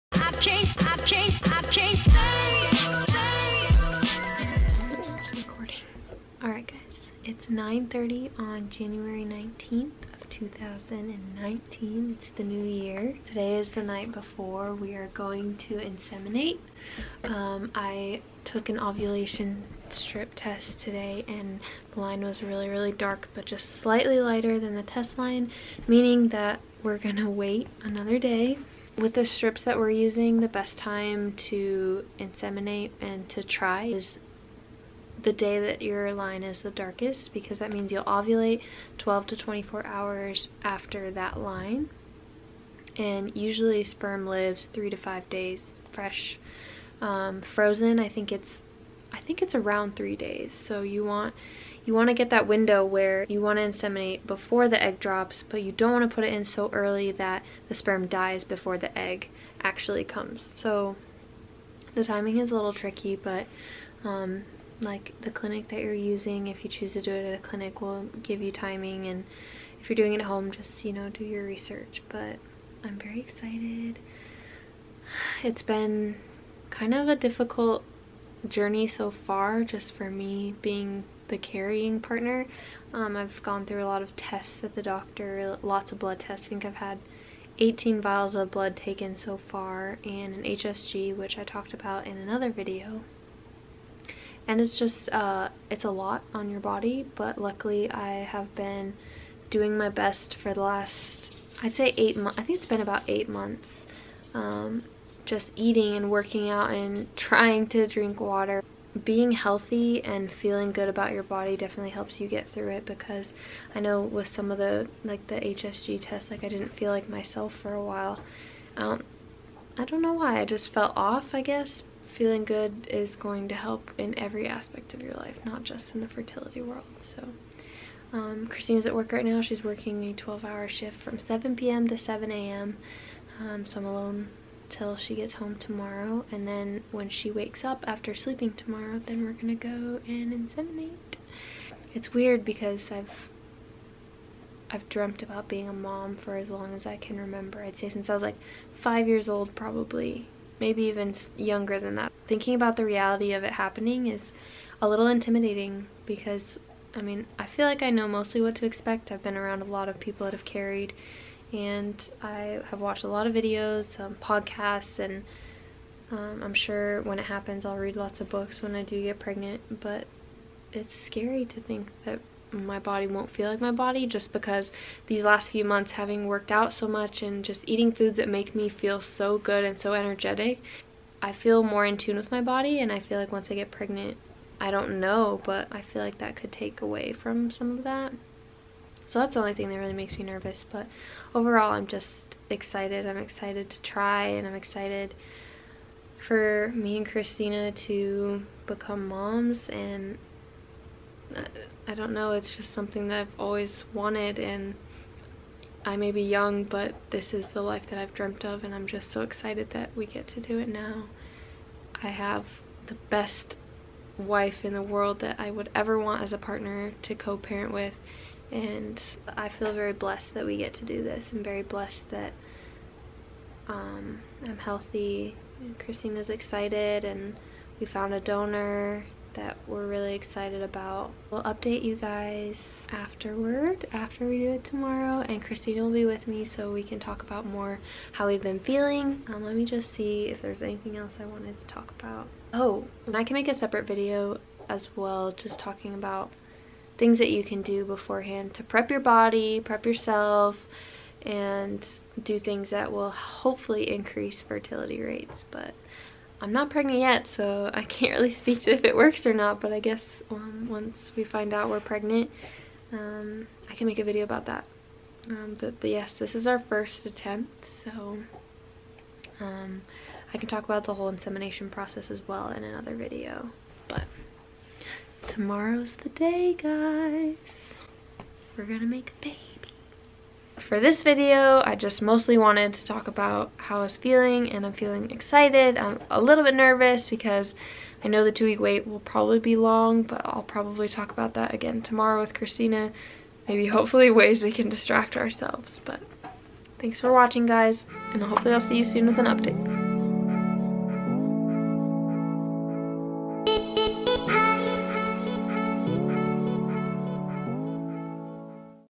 Lesbian TTC Journey | Ep. 2 | We're About to Inseminate!
In this video I talk about a lot of the feelings I'm experiencing as we prepare to do our first insemination. I hope if you're watching this and going through something similar, that you're able to keep a positive mindset and always remember the end goal!